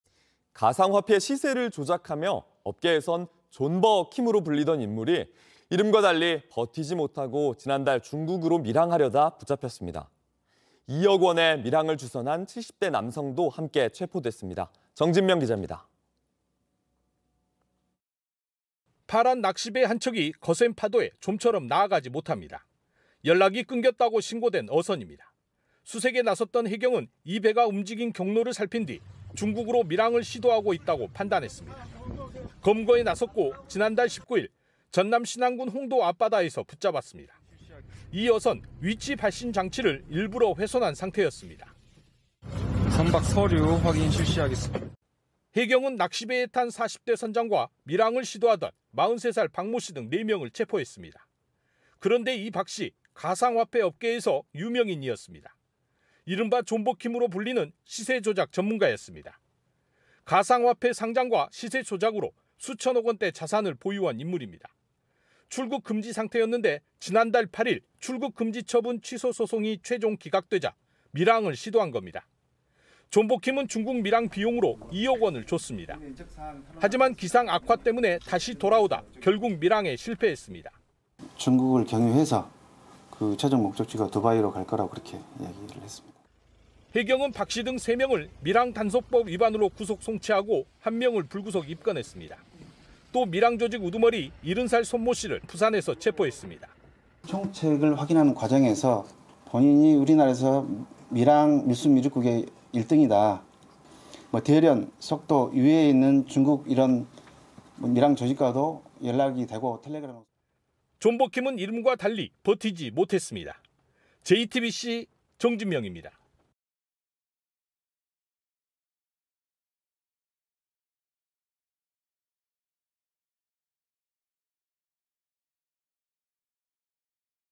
가상화폐 시세 조작 전문가 '존버 킴' 검거…기상악화로 밀항 실패 / JTBC 아침&
가상 화폐 시세를 조작하며 업계에서 '존버 킴'으로 불리던 인물이 이름과 달리 버티지 못하고 지난달 중국으로 밀항하려다 붙잡혔습니다. 2억 원에 밀항을 주선한 70대 남성도 함께 체포했습니다.

정진명 기자입니다.